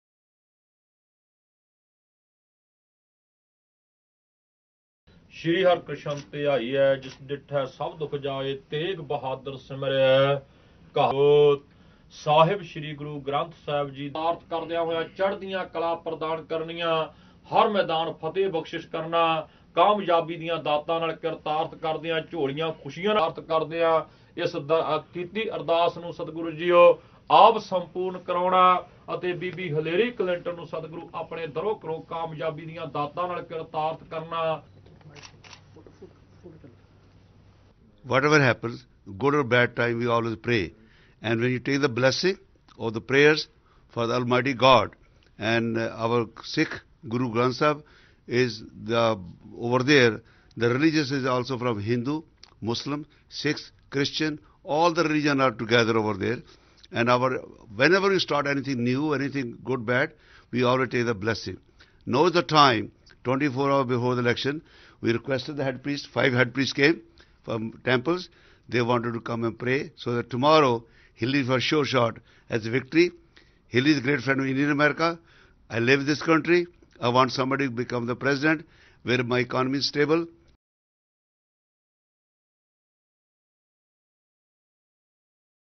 U.S. polls: Indian American businessman Chatwal offers ardas for Hillary Clinton’s win
New York, Nov : Indian American businessman Sant Singh Chatwal on Monday offered special ardas for Democratic Presidential candidate Hillary Clinton's victory in the election to be held tomorrow. Clinton is contesting against Republican candidate Donald Trump.